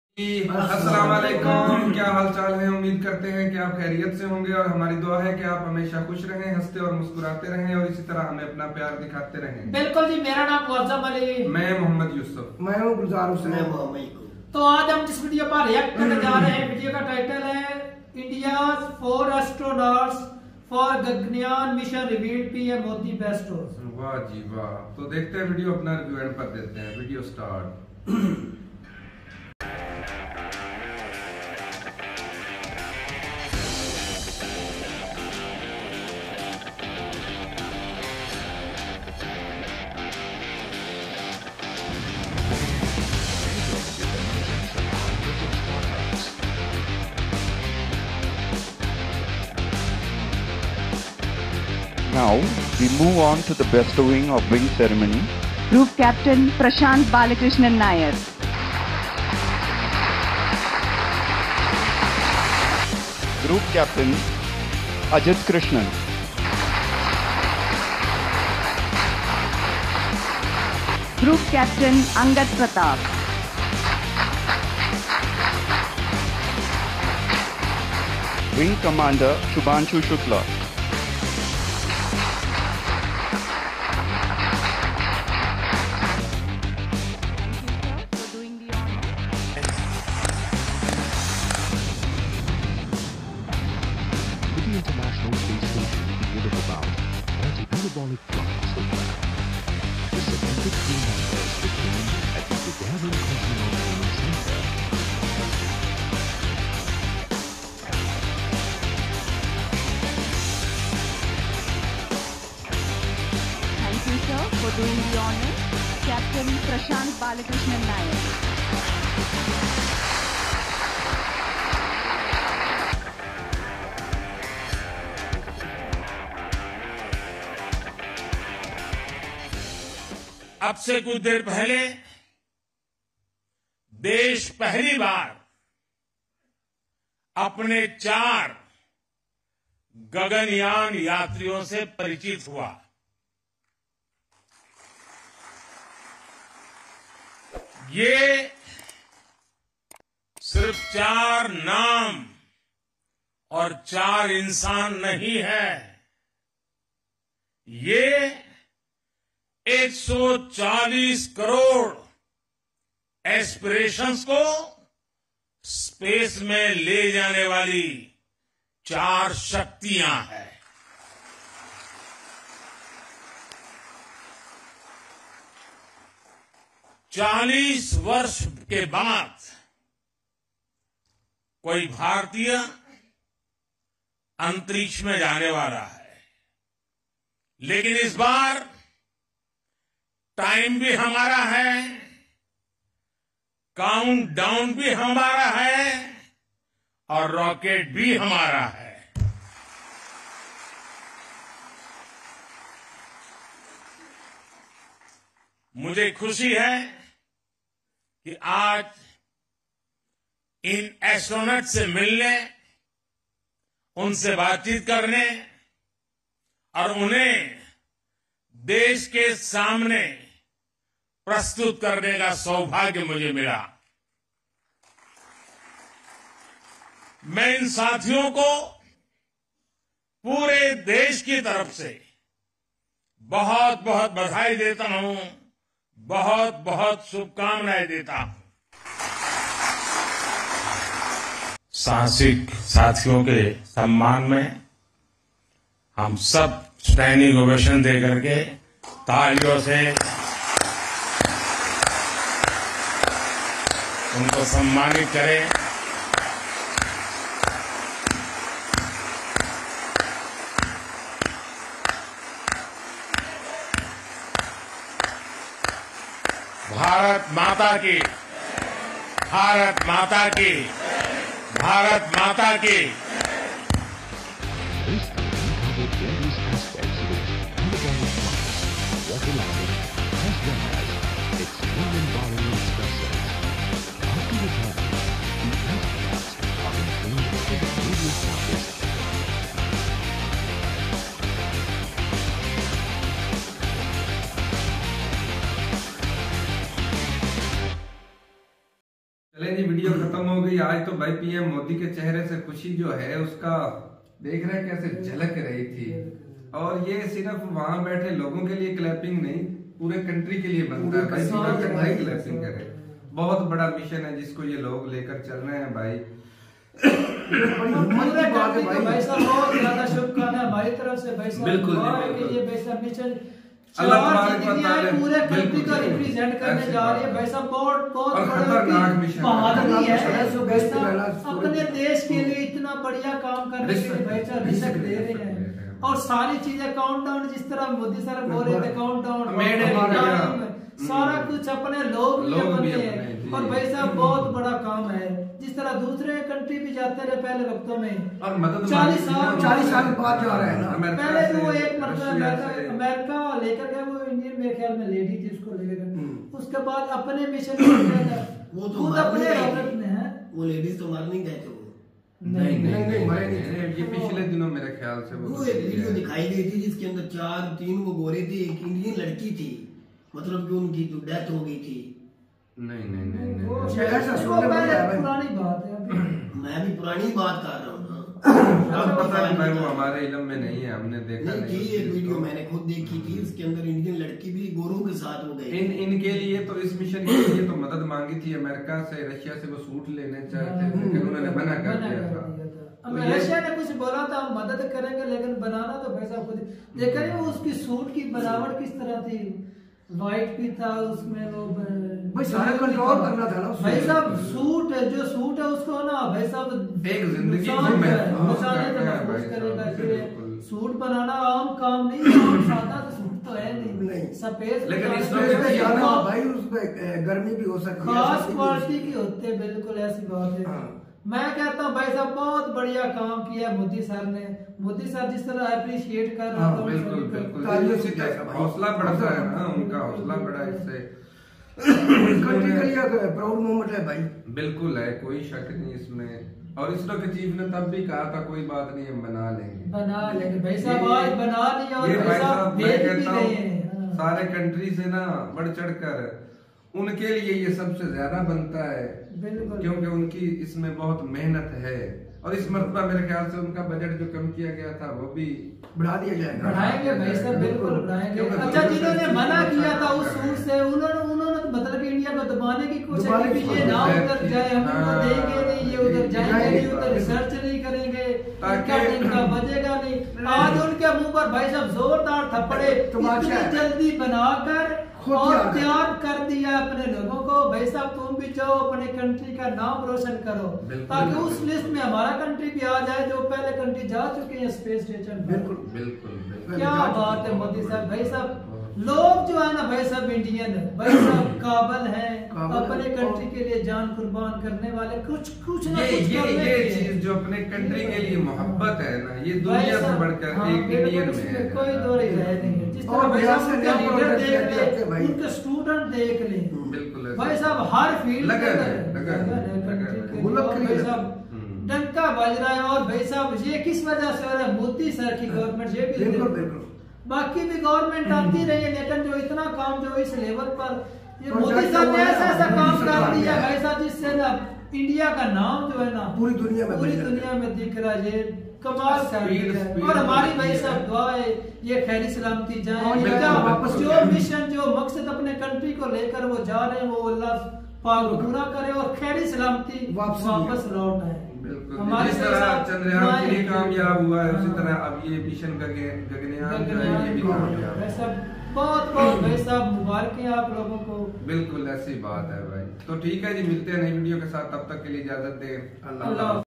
India's 4 astronauts forGaganyaan mission revealed,PM Modi bestows astronautwings
India's 4 astronauts for
Gaganyaan mission revealed,
PM Modi bestows astronaut
wings